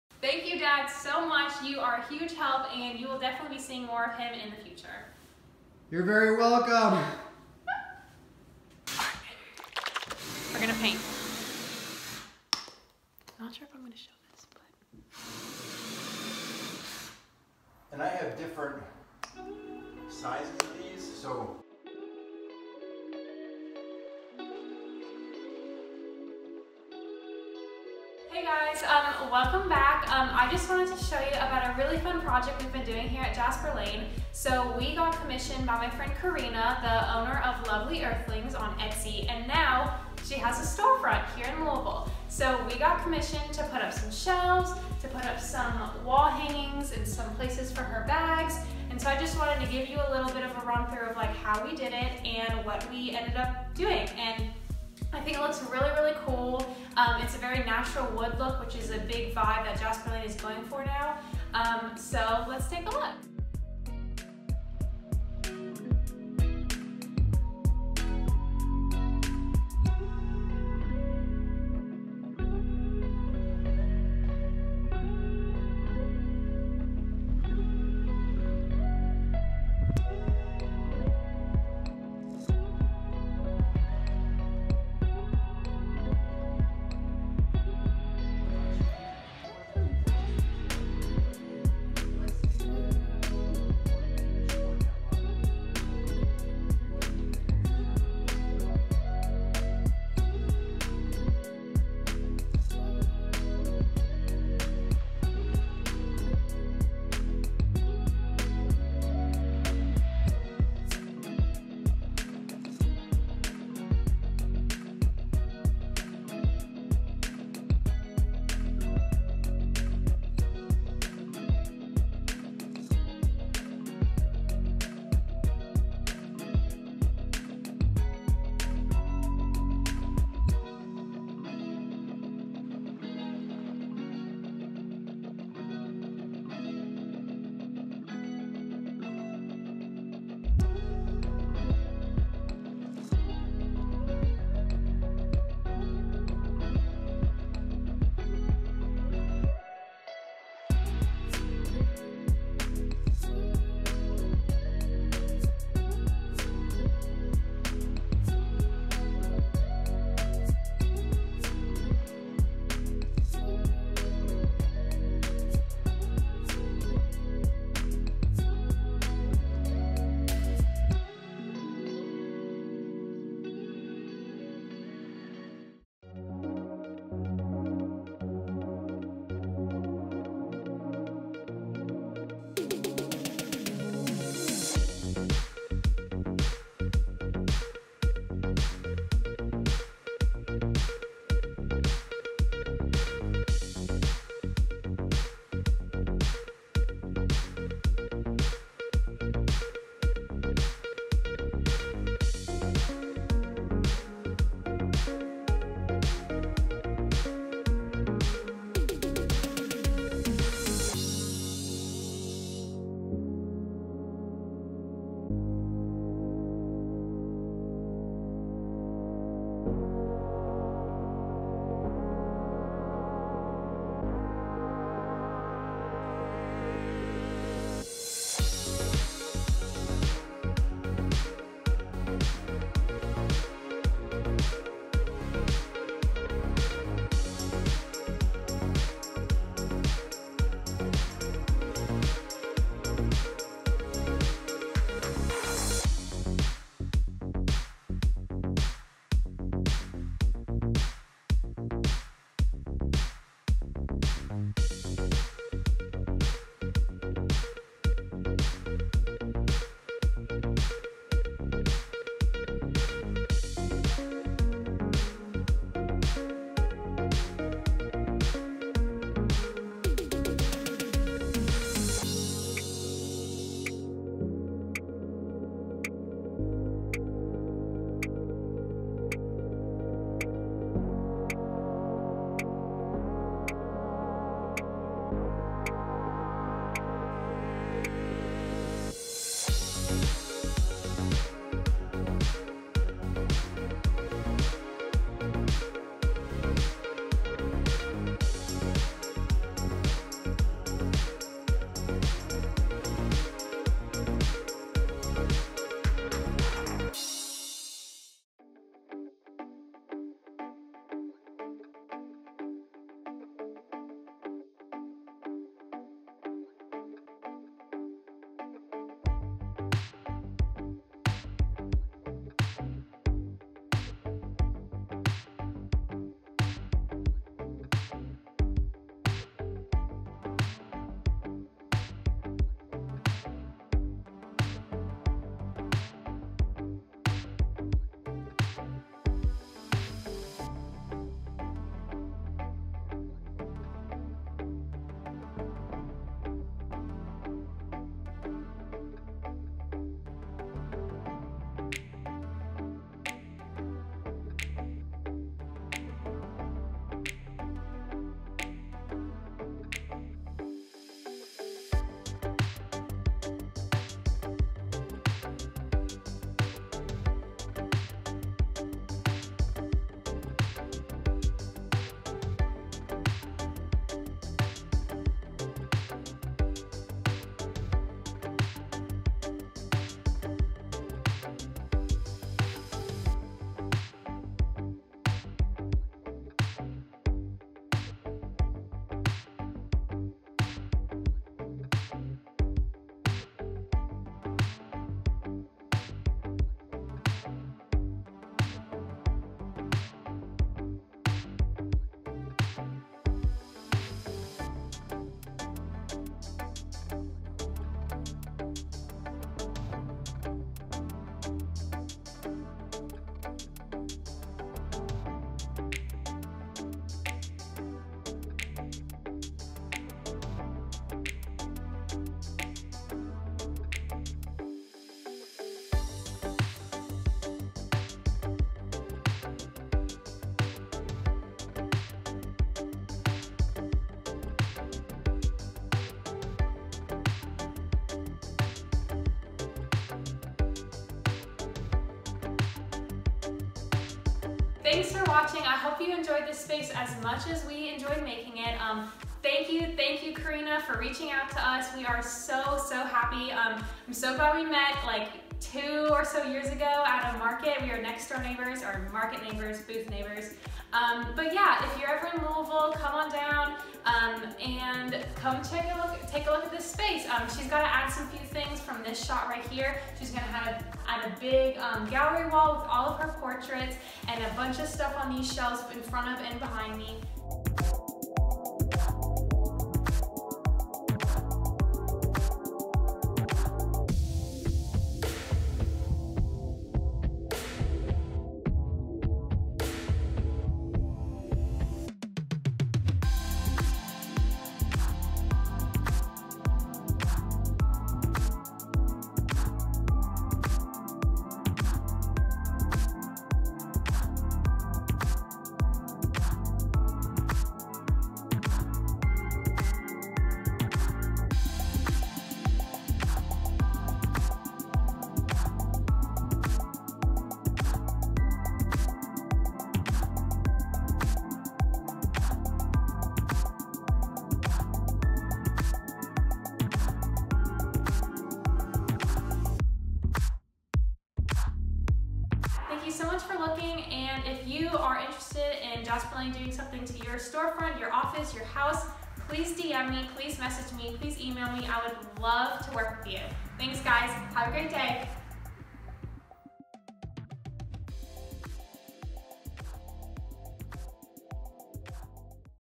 Storefront makeover | lovely earthlings
So happy for my friend Karina with Lovely Earthlings for having her own storefront!!  Thank you for reaching out to Jasper Lain to help you create your dream space!! Watch to see the process of creating this lovely space!

follow Lovely Earthlings
etsy- lovely earthlings

HUGE thank you to my dad for helping again with this project! Could not have created any of this without you.

Thank you to my editor Bella DeLain. You can follow her on Instagram or here on YouTube!